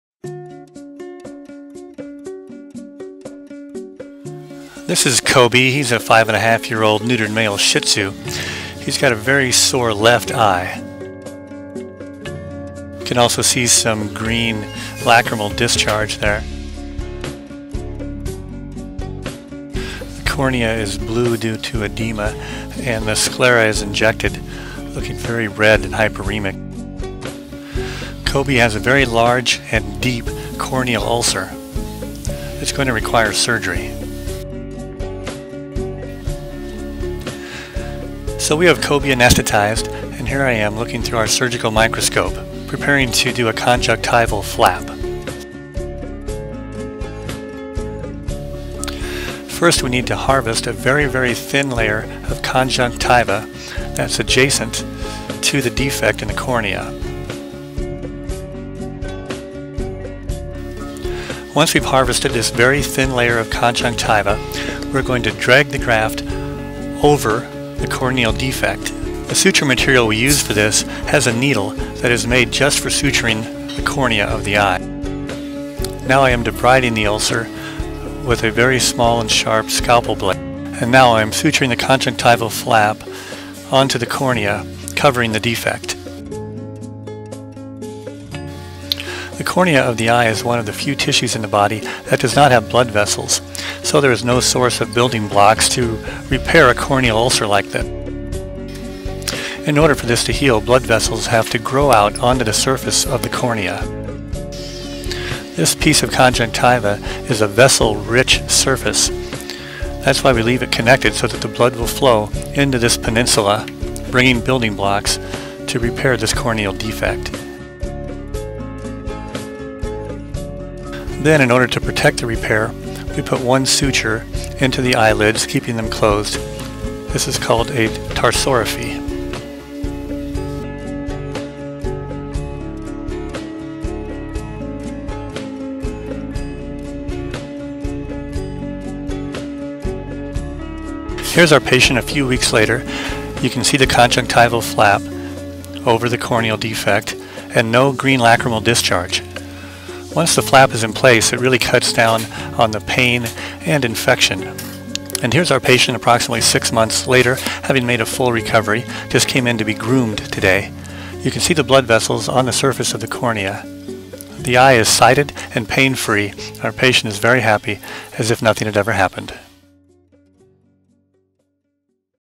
Koby has a severe corneal ulcer
We treat a severe corneal ulcer with a conjunctival flap.